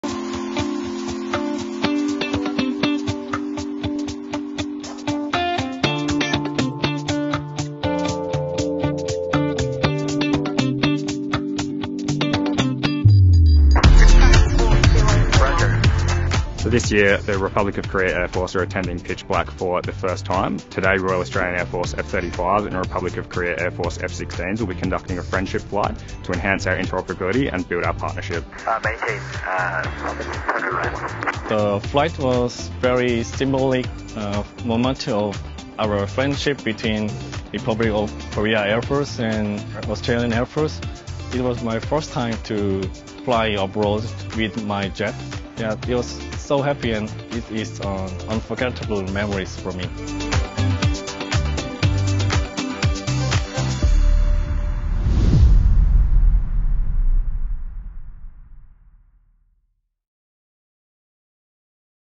Exercise Pitch Black 2022 | Friendship flight with Republic of Korea Air Force 🇰🇷🇦🇺
Unforgettable memories and fast fighter friendships forged. ✈️ 💪

This year we welcomed the Republic of Korea Air Force (ROKAF) to Exercise Pitch Black for the very first time.

Our F-35A Lightning II pilots flew a friendship flight with their KF-16U Viper crews that helped us to build our collective knowledge, and lasting professional relationships.

And subsequent to the exercise Air Vice-Marshal Darren Goldie, Air Commander Australia and Lieutenant General Park Ha Sik, Commander ROKAF Operation Command have signed an arrangement formalising air-to-air refuelling cooperation between our two air forces. Extending—literally!—how we can support one another in the skies.

Through training exercises like PitchBlack22, we enhance our interoperability and partnerships with international forces.